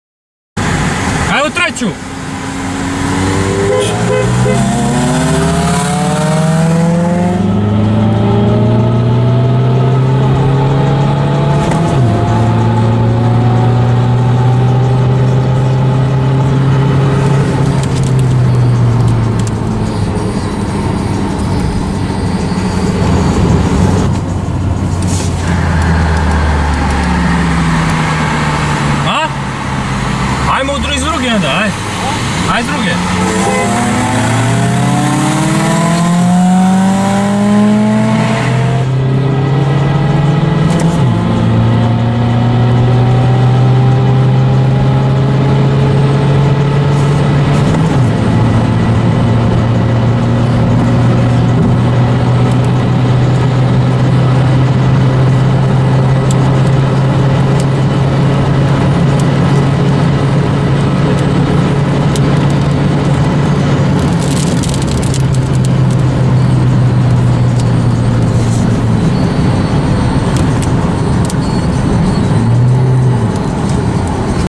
VW GOLF 3 GTI 2 0 vs AUDI A4 B7 AVANT
GOLF 3 GTI 2 0 150HP vs AUDI A4 B7 AVANT 2.0 160 HP
 Weight : 1139 kg 1600kg
FOLLOW VR6 TURBO ON INSTAGRAM